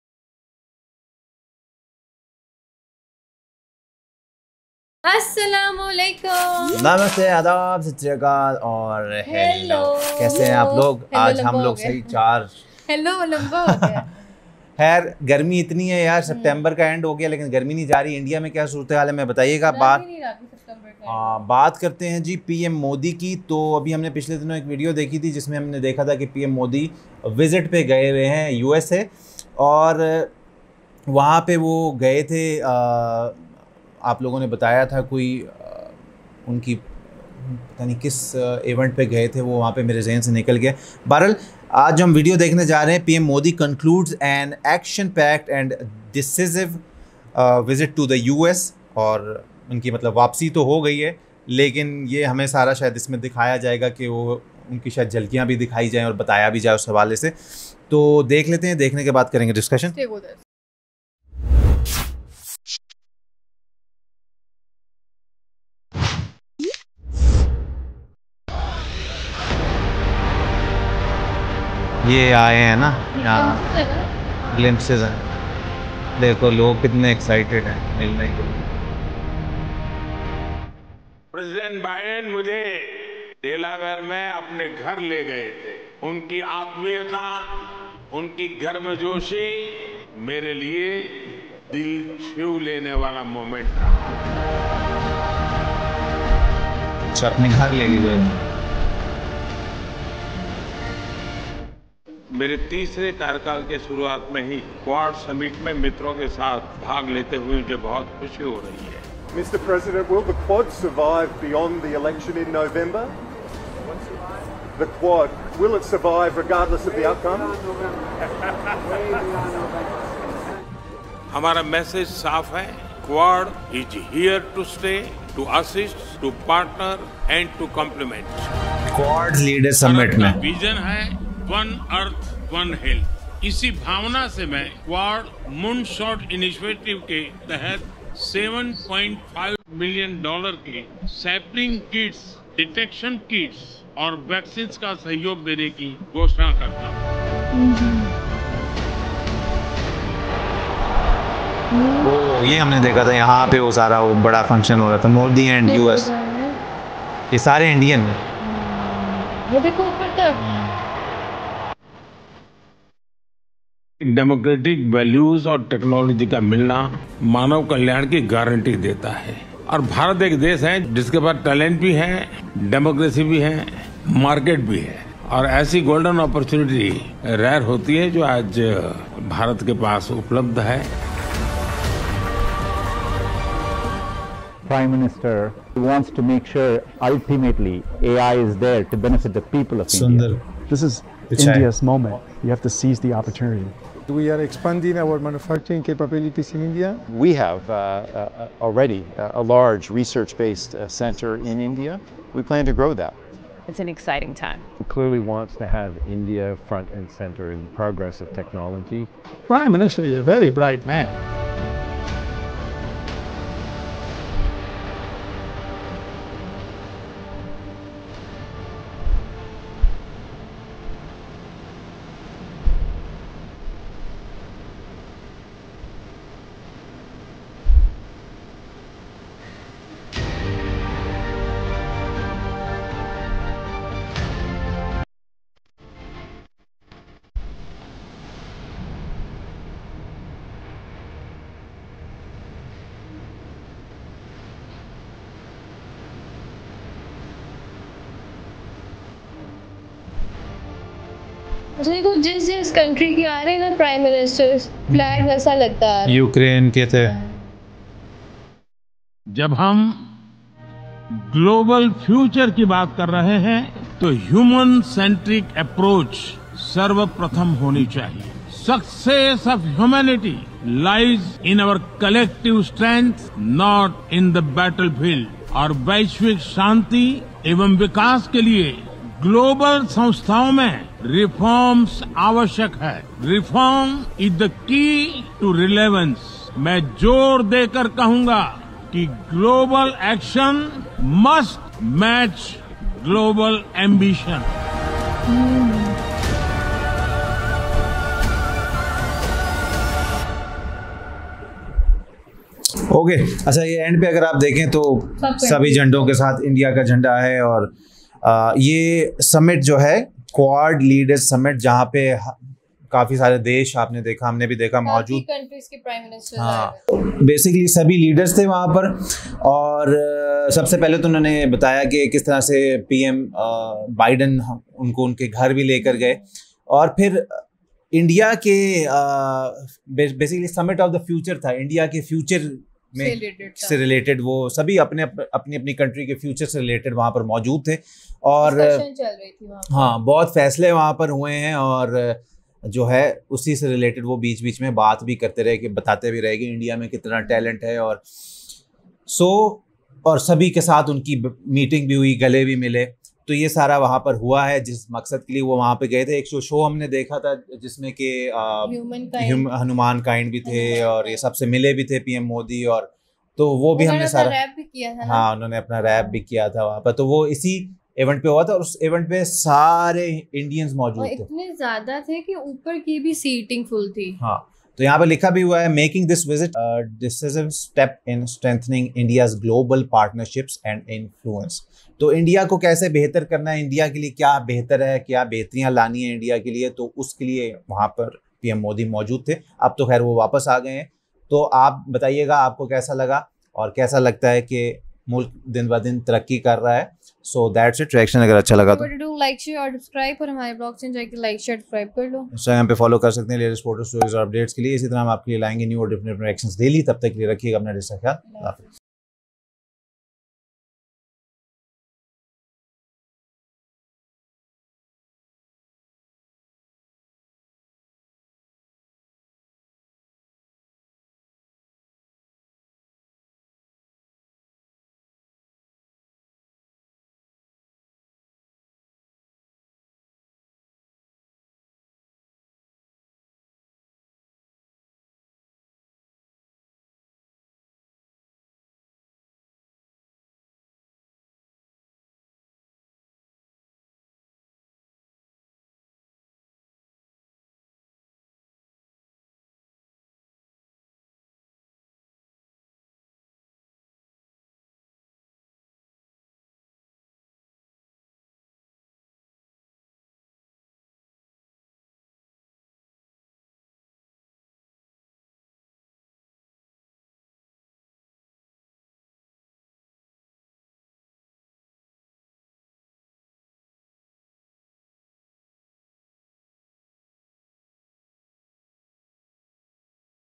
Pakistani Couple Reacts To PM Modi Concludes Action Packed & Decisive Visit To The US
Credits :
Channel Name : Narendra Modi